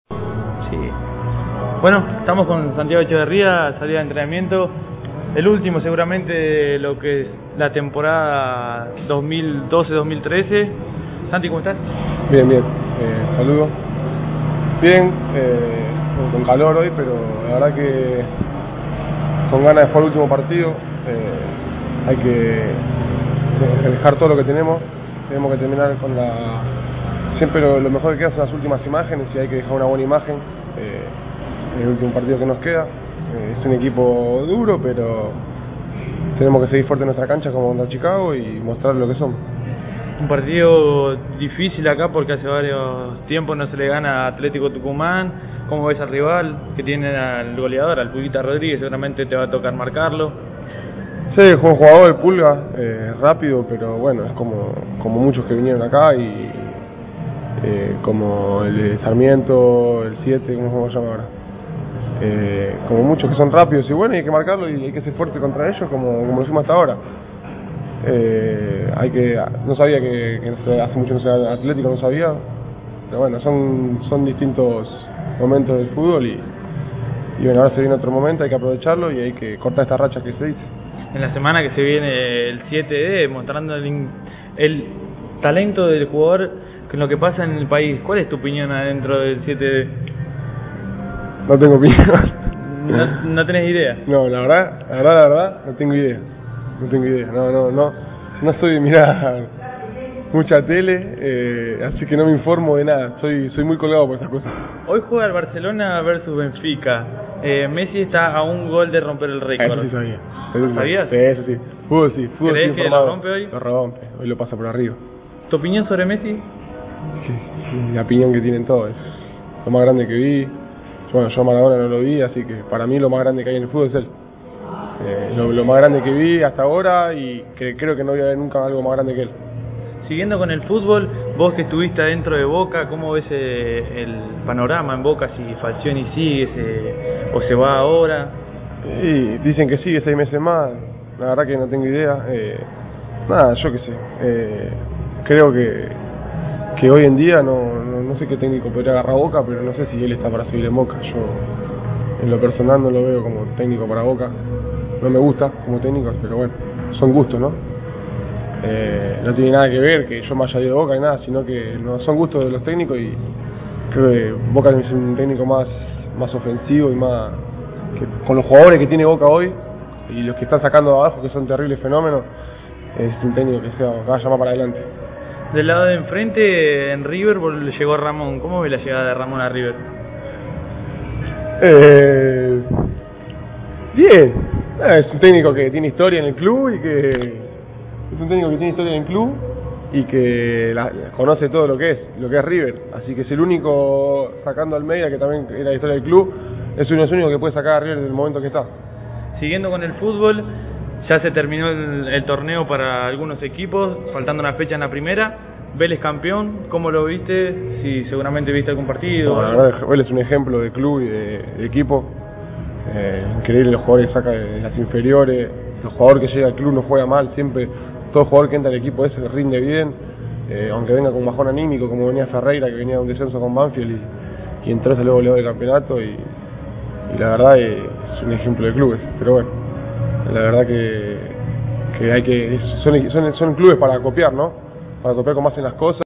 Nota a Santiago Echeverria, al finalizar el entrenamiento. 06/12/2012
Nota : Juan I. Sayavedra.